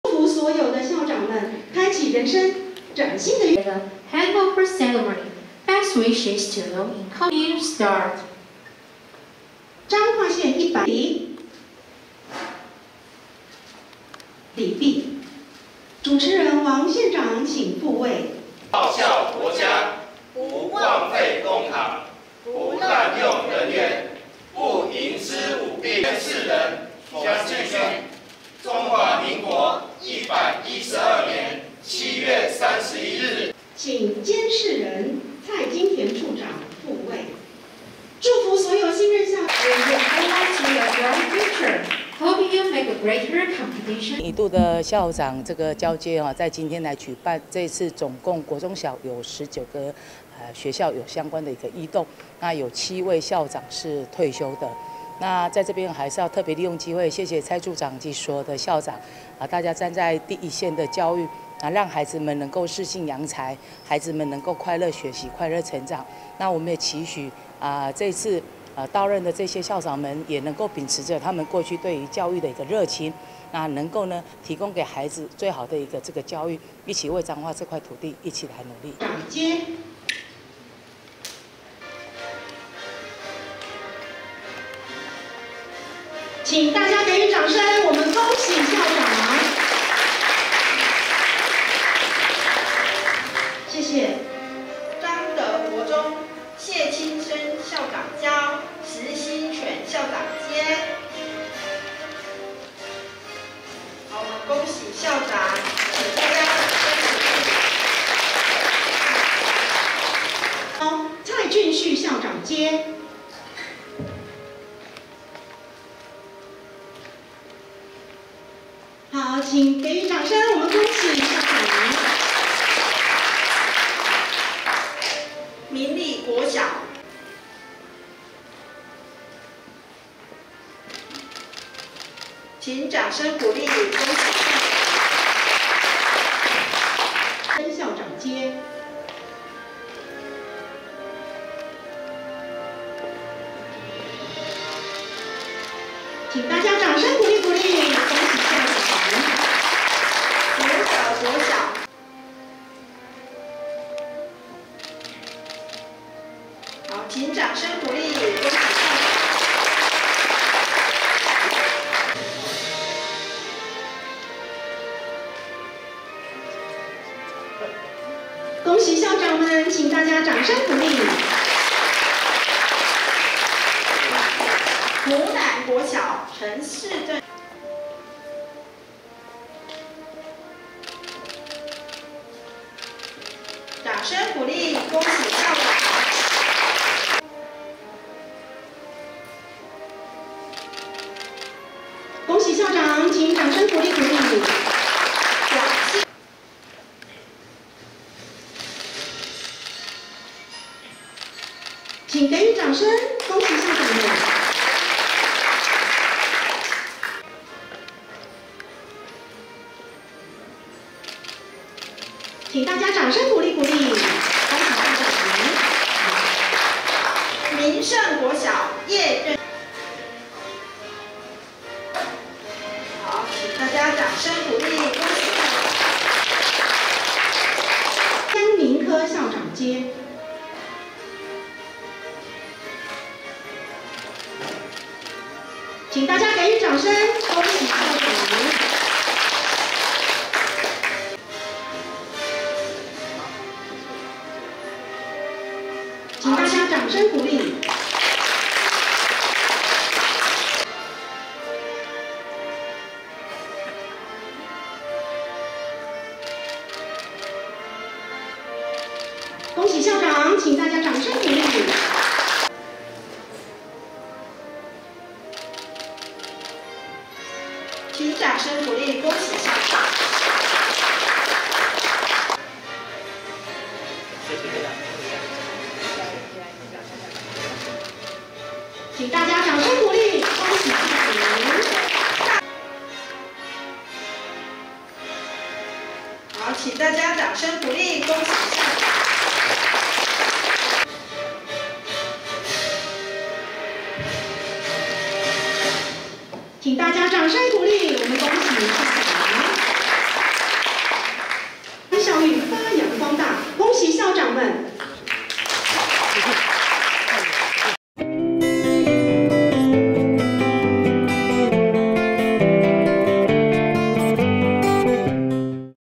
彰化縣112學年度中小學校長交接 杏壇交棒薪火相傳
彰化縣112學年度中小學校長交接　杏壇交棒薪火相傳 【記者林明佑／彰化市報導】彰化縣112學年度中小學校長交接典禮，31日在民生國小仁愛樓音樂廳舉行，由縣長王惠美主持，典禮計有國中小等19校辦理校長交接，教育處長蔡金田擔任監誓人，主計處長蕭秀瑛擔任監交人，王惠美除感謝所有校長對教育的用心付出，讓彰化的教育更美好，也期勉所有校長，未來一秉教育的熱情，提供孩子們最好的學習，為彰化這塊土地一起努力。@changhuanews